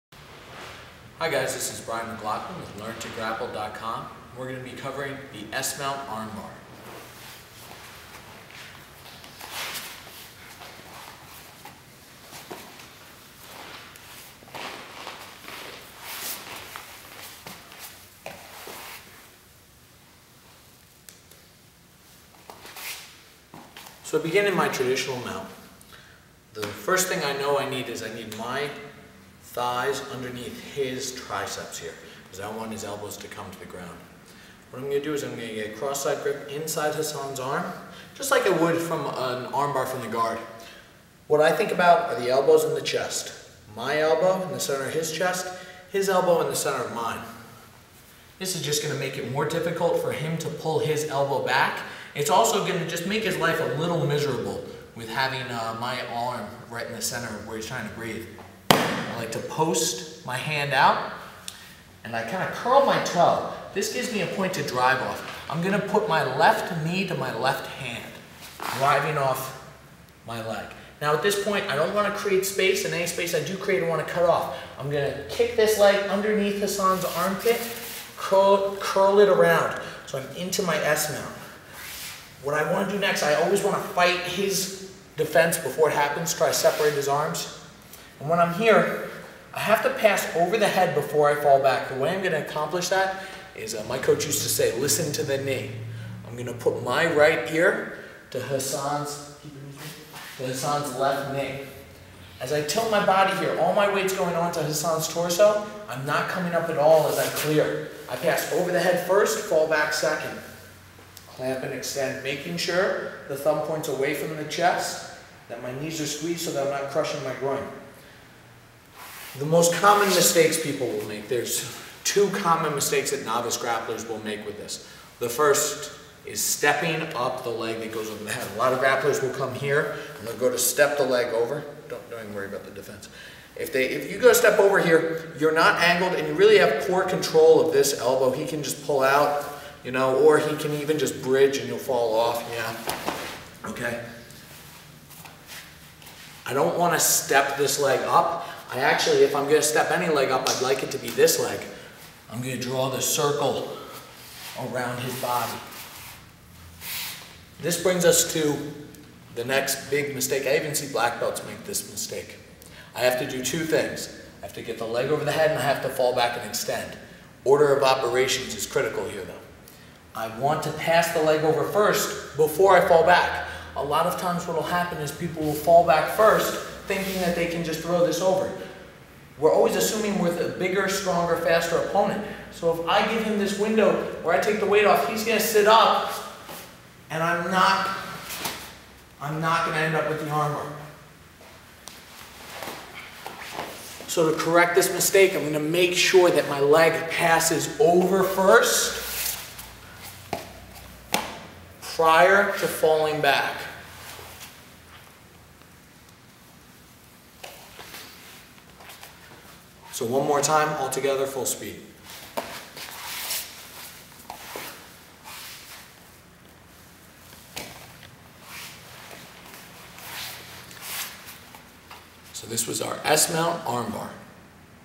S Mount Armbar - Learn to Grapple
BJJ Blackbelt Brian Mclaughlin shows the s mount armbar.